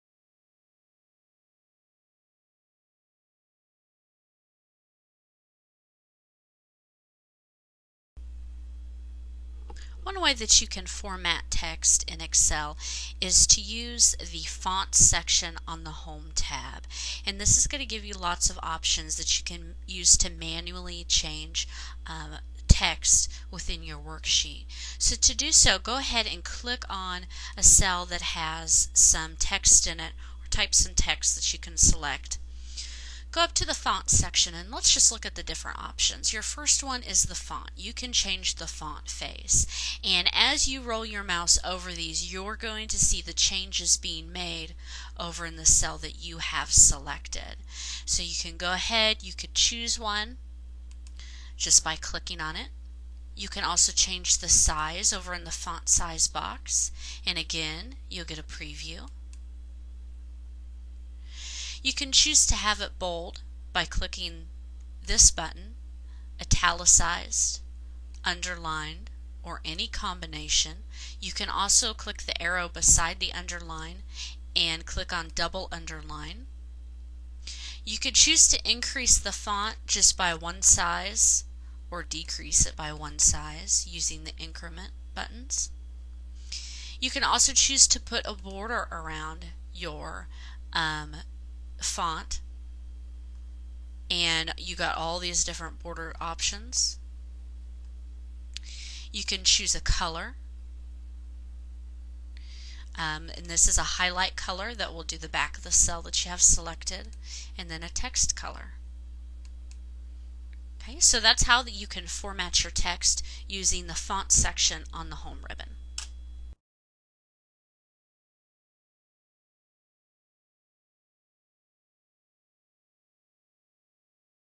Format Cell Text in Excel 2007
See a higher quality video on TechTutor.TV! Text entered directly into cells needs to be formatted so it can look its best and direct users in the right direction to see the data you are directing them to. In this tutorial, you will learn how to format cell text in Excel 2007. See higher quality tutorials all for FREE